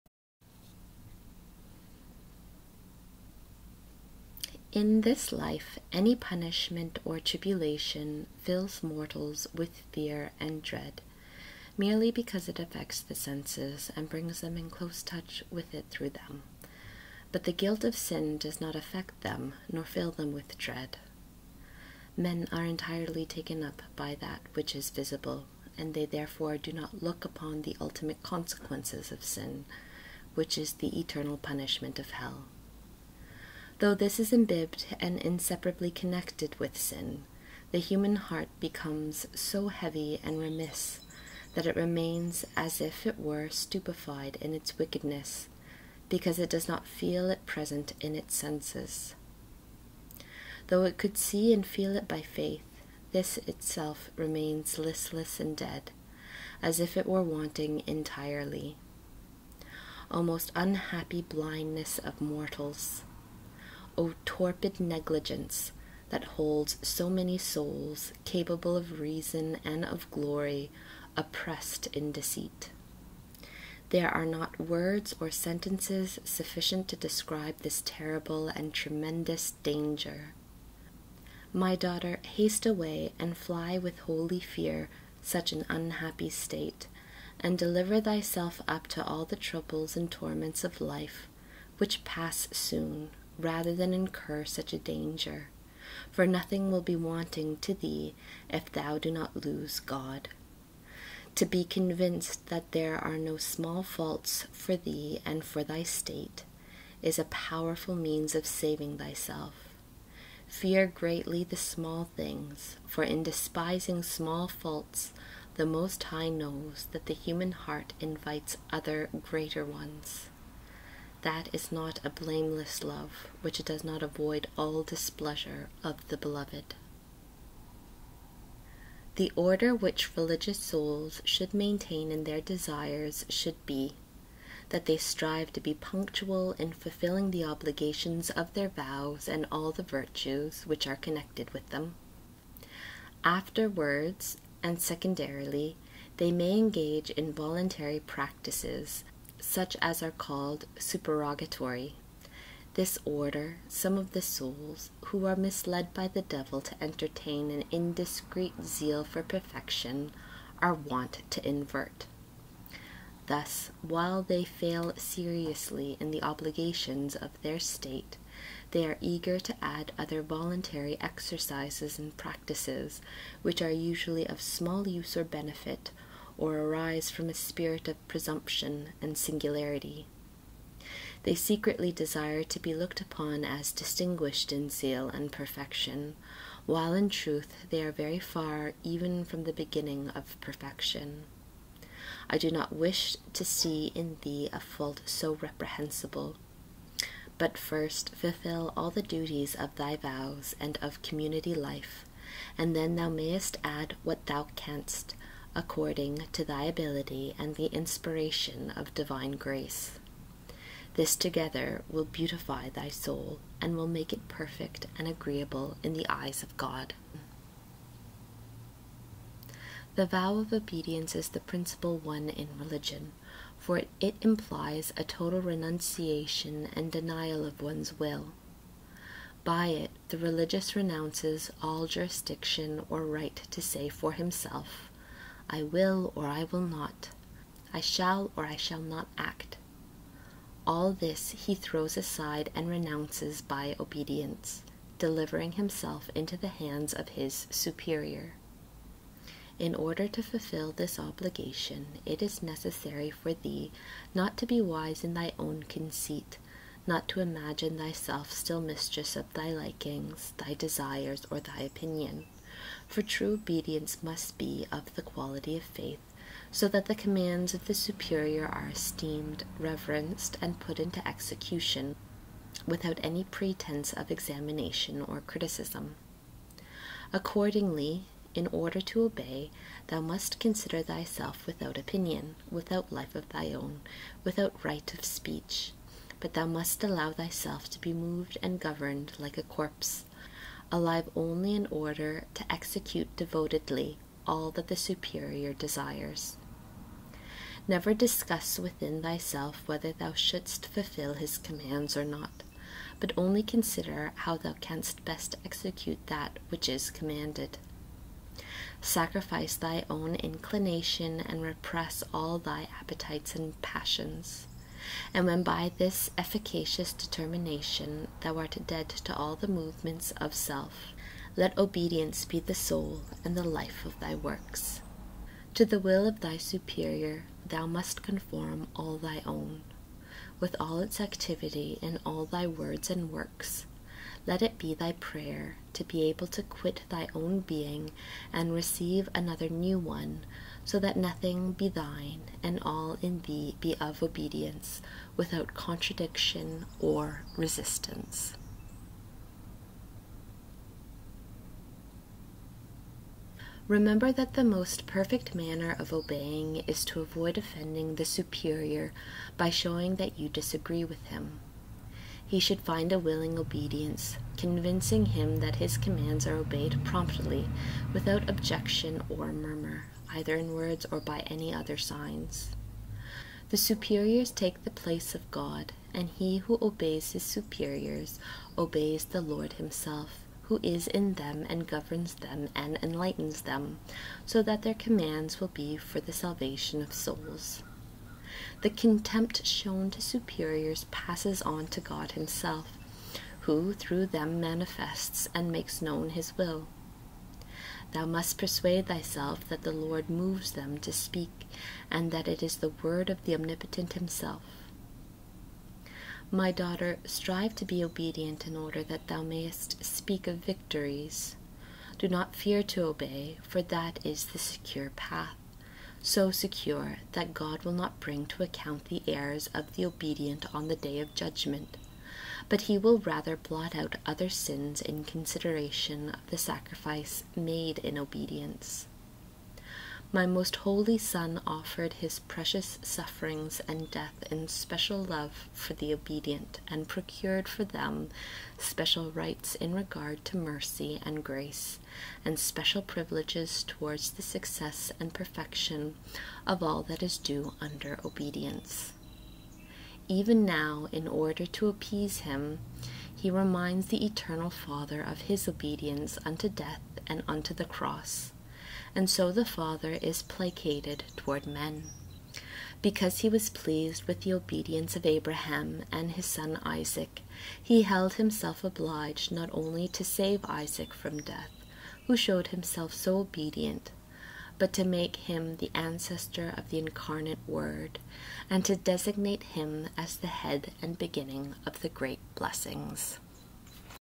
Mystical City of God - Book 2 - Chapter 3 (Part 2/5)
Image - The Pharisee and the Publican by Barent Fabritius

Paragraphs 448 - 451 of Book 2 of the Mystical City of God by Mary of Jesus of Ágreda.